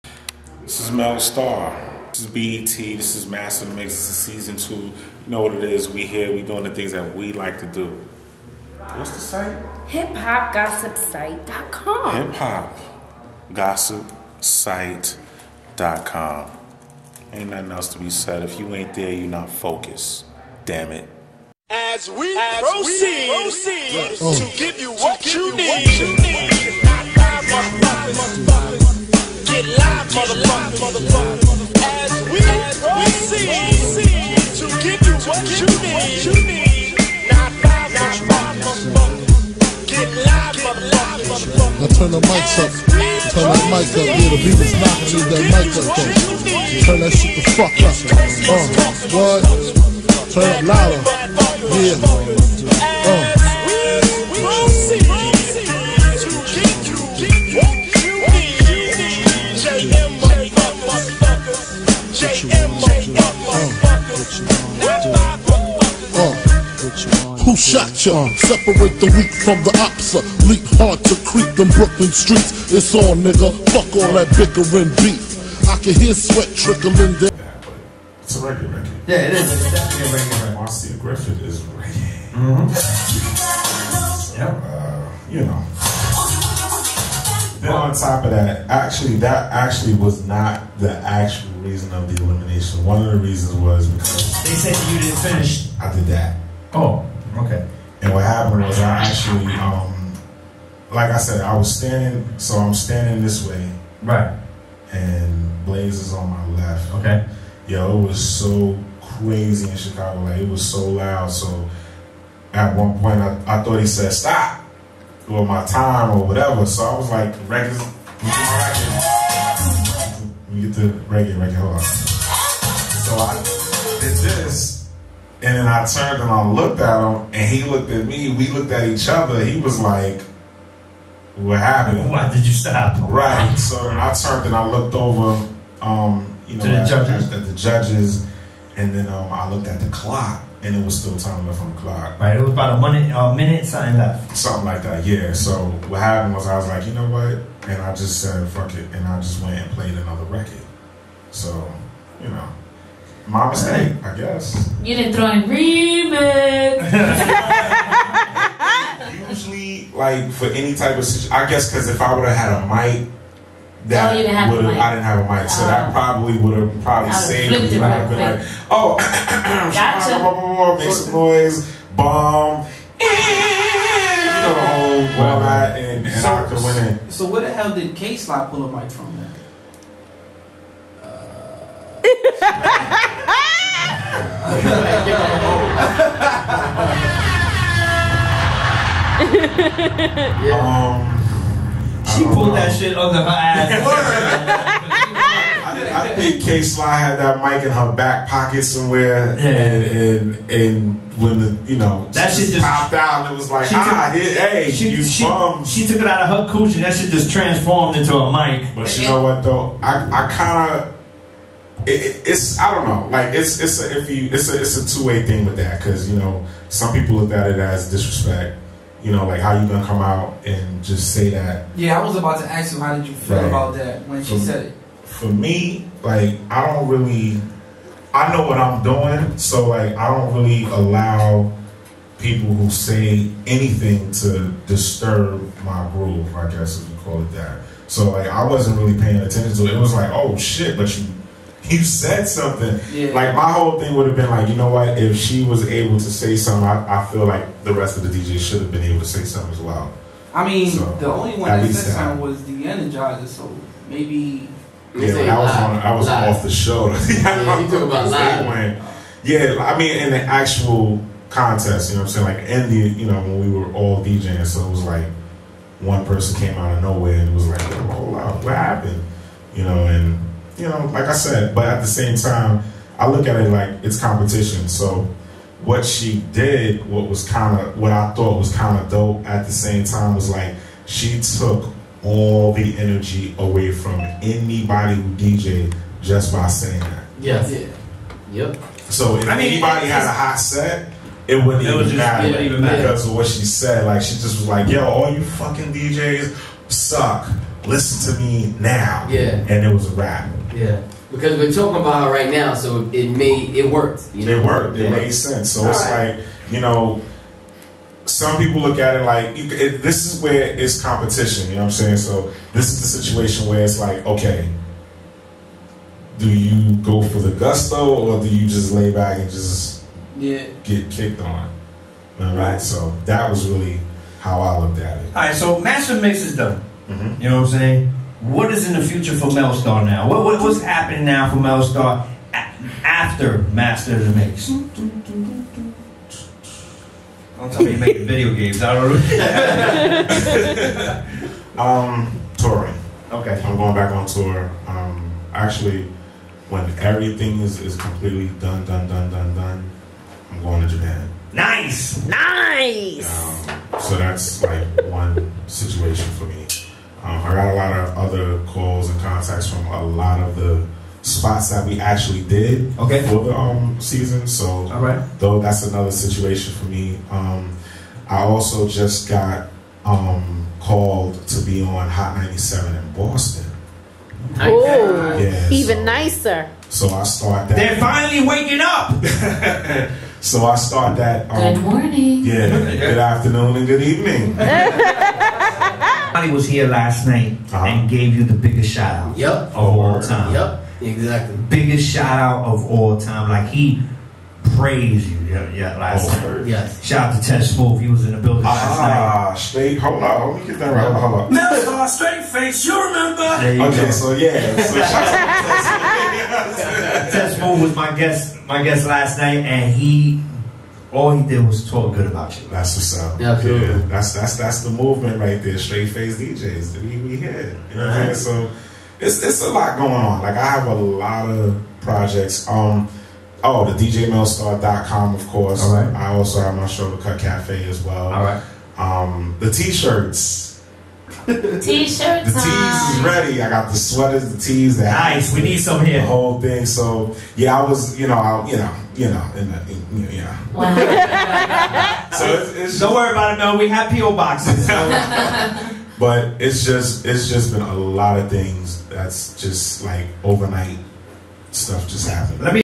DJ Mell Starr Talks Elimination on BET's Master Of The Mix!!!
DJ Mell star was one of the contestants to be featured on BET's "Master of the Mix" reality show. The judges of the show are DJ Kid Capri, Amber Rose and Vikter Duplaix and unfortunately, Mell Starr was eliminated week before last. So find out what Mell Starr had to say about his loss here or on HipHopGossipSite.com!